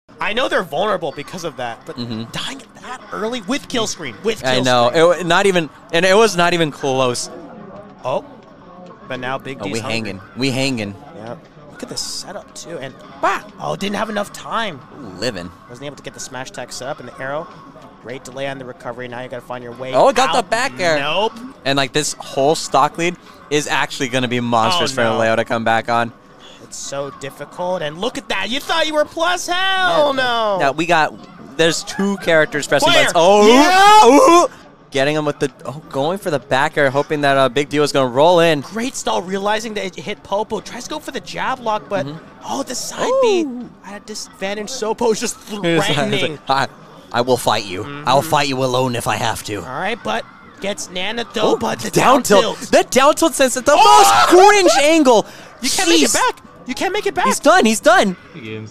BIG D DOMINATES MKLEO GAME 5 - SHARK TANK #61 HIGHLIGHTS
Big D is broken!